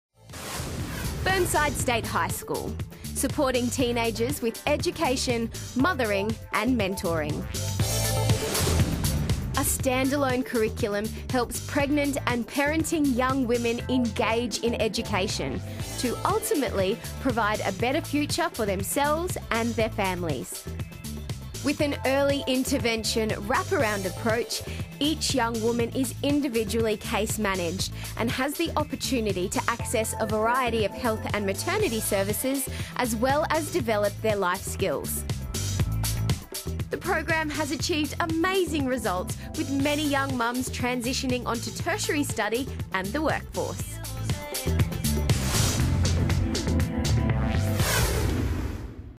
Burnside State High School -- Winner 2012 Showcase Award for Innovation
Burnside State High School
Supporting teenagers with education, mothering and mentoring (STEMM)

By educating a mother, you educate a family. This philosophy underpins the highly successful STEMM program that links education, Queensland Health, community and industry.

This unique program supports and empowers pregnant girls and young mothers through an innovative stand-alone curriculum. STEMM provides pathways to achieve certified outcomes, as well as personal and parenting support, mentoring and community access.

The wrap-around service model is based on best practice research and is a key strategy to re-engage pregnant and parenting teens with education to gain independence for themselves and improve their children's futures.